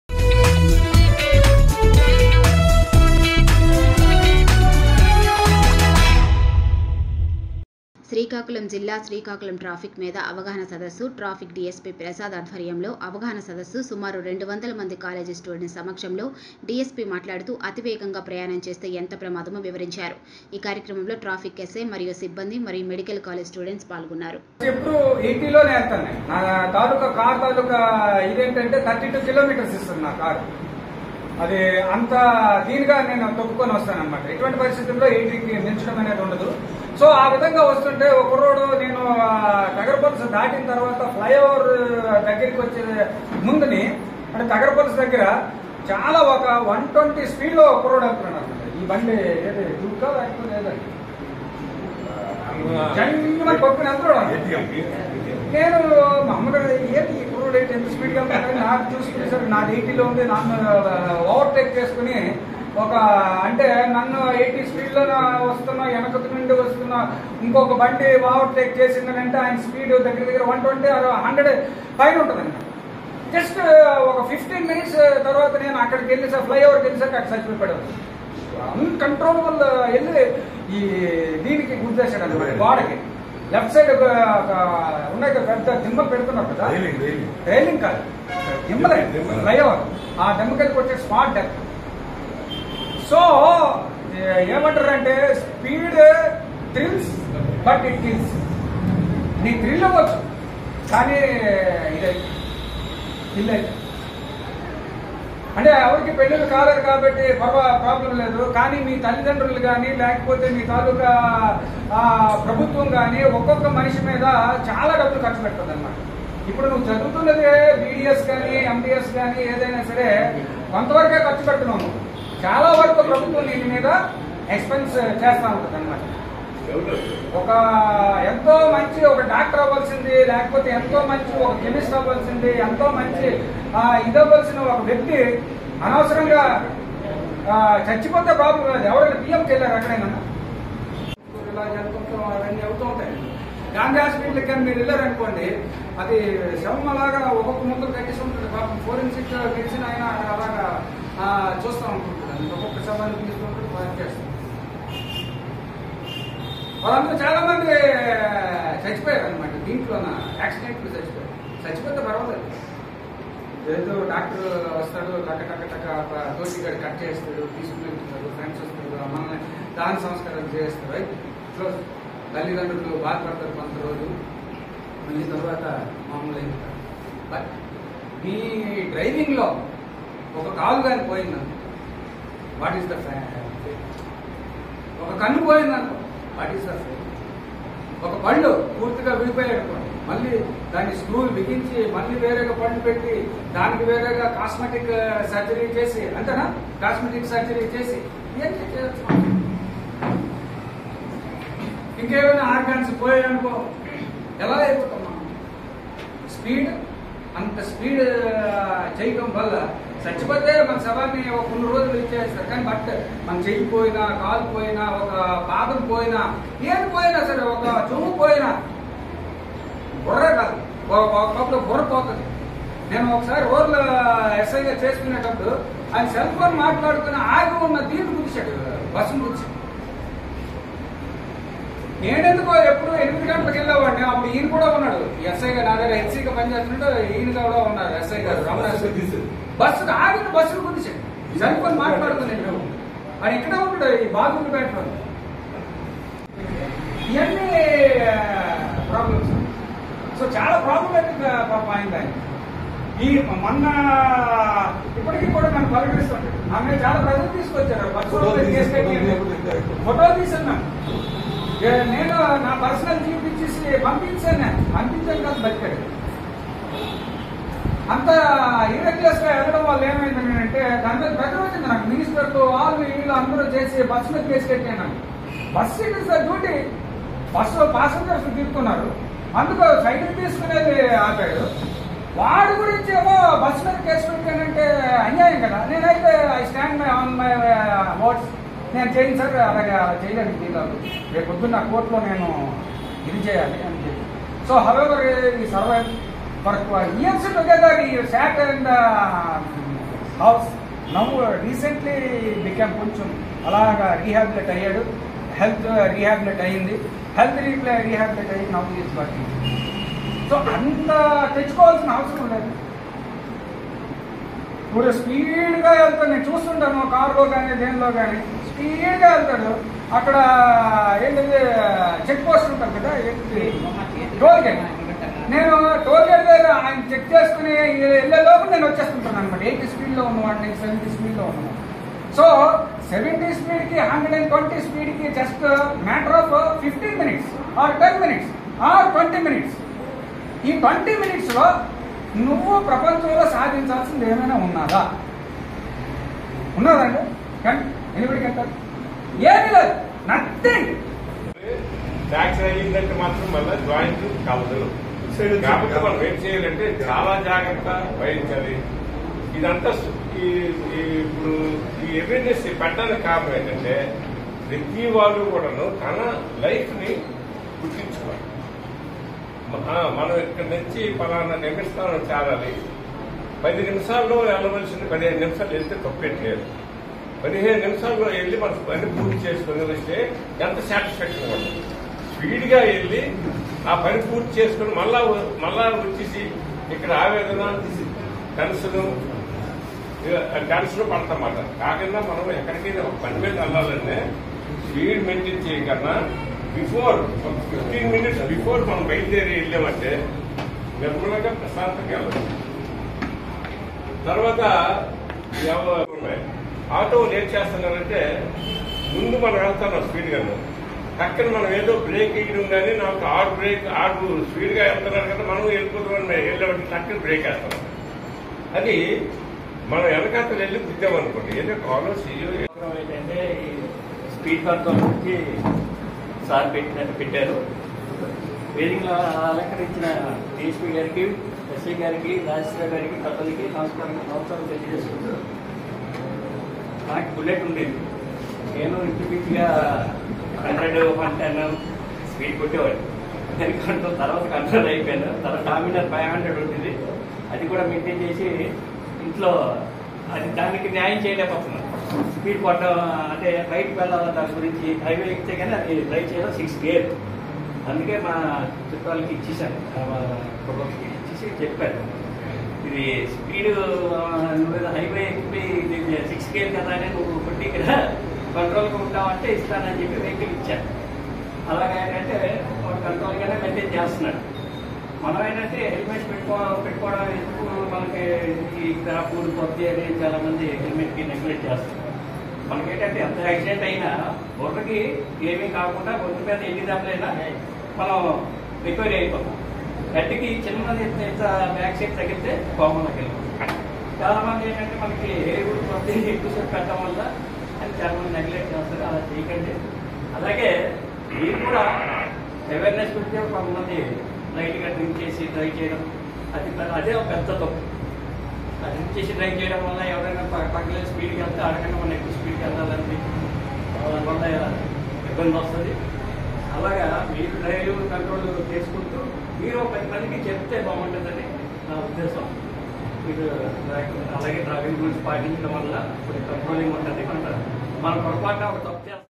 శ్రీకాకుళం|| డిఎస్పి ప్రసాద్ గారు ఆధ్వర్యంలో ట్రాఫిక్ మీద అవగాహన సదస్సు
@truthnewstelugu శ్రీకాకుళం|| డిఎస్పి ప్రసాద్ గారు ఆధ్వర్యంలో ట్రాఫిక్ మీద అవగాహన సదస్సు